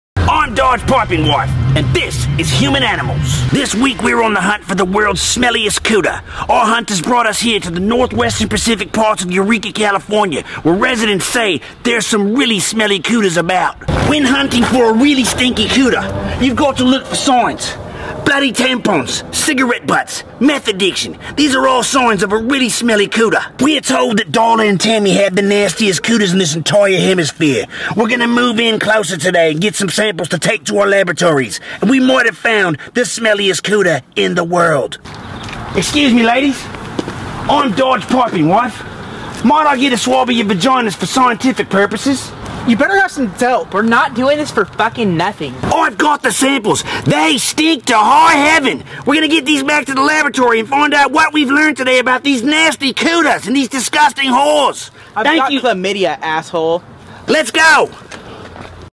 BLACKMARKETVIKINGS : SMELLY COOTER SAFARI
Humboldt County local tour guide, Dodge Pipingwife, spearheads a safari in search of the worlds smelliest cooter. The hunt was a success.

🐐’S x 10 !!

To anyone interested in collabs, product reviews, becoming an official sponsor and/or proposing a network deal - please email our Business Manager